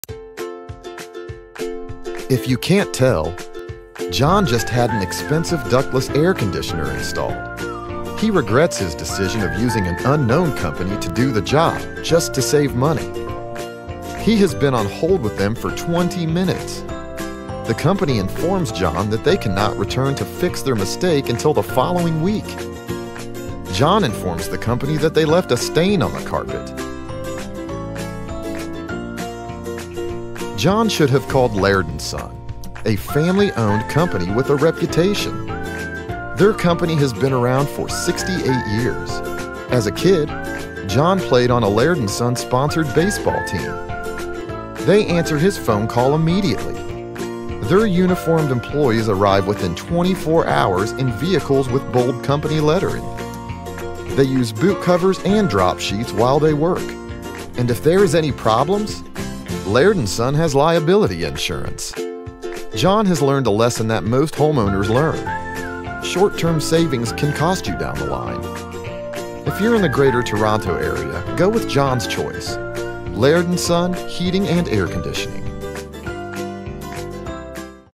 This is a Story about John... choosing Laird and Son
John had an expensive ductless air conditioner installed, but hired a no-name company to install it. He was trying to save money and regretted the decision.

Laird and Son have been around for 68 years and are a reliable company that specialize in all areas of HVAC.

If you want to have a ductless air conditioning unit installed, please contact a Laird and Son ductless specialist @ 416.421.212.